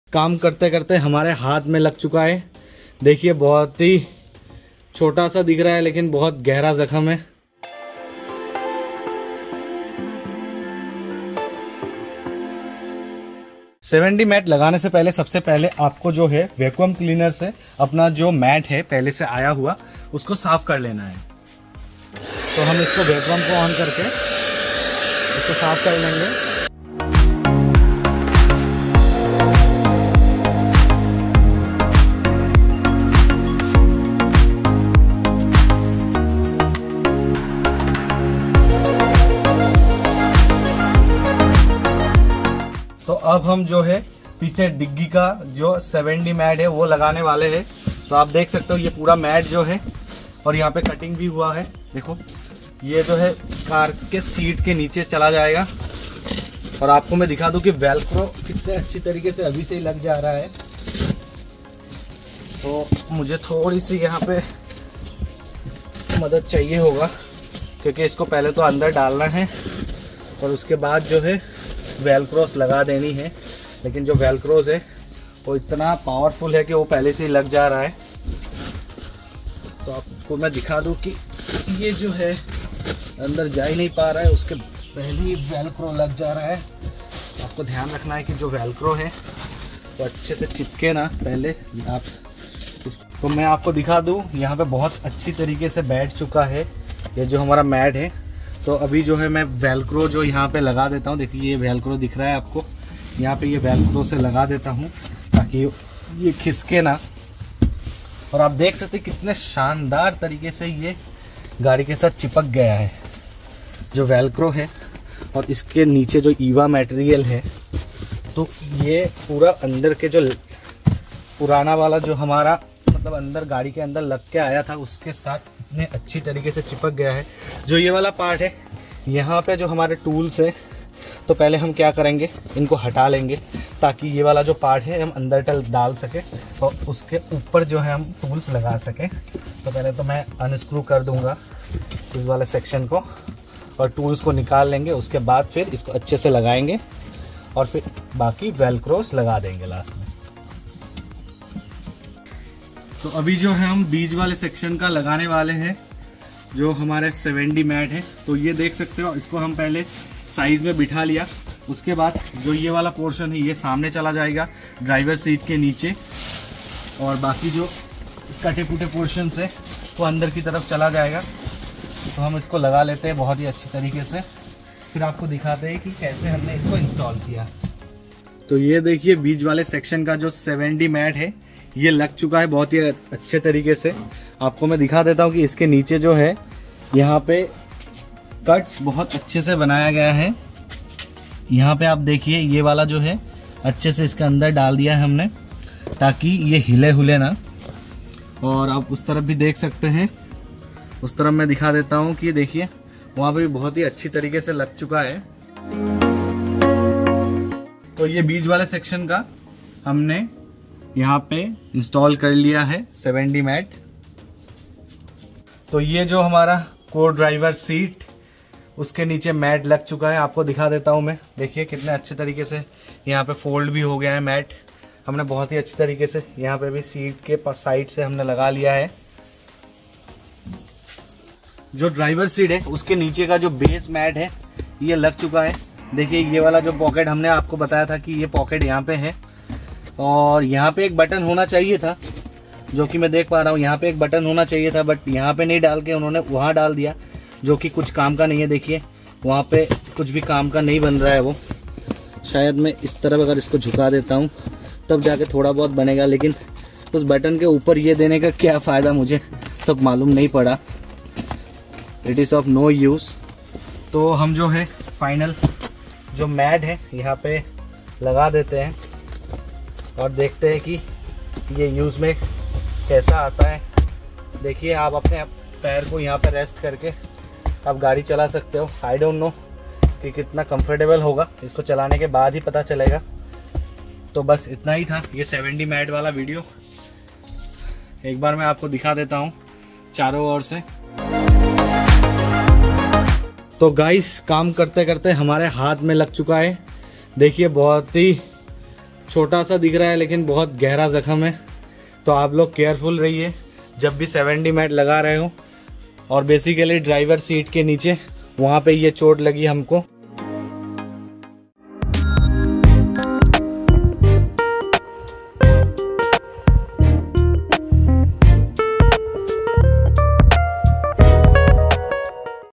7D MAT INSTALLATION IN BOLERO NEO- LEARN HOW TO INSTALL SEAT COVER IN YOUR CAR 🚘✌🏻
All Vehicle's Car Mat and Seat Cover Is Available :: 3D 4D 5D 6D 7D Mat and GFX Mat

BEST BUDGET 7D CAR MAT- SEAT COVER- FOR YOUR VEHICLE- BOLERO NEO 7D CAR MAT

7D MAT INSTALLATION IN BOLERO NEO- LEARN HOW TO INSTALL SEAT COVER IN YOUR CAR

You can follow my elder Brother who made this video possible 👇🏻
Kaumarjya Mandal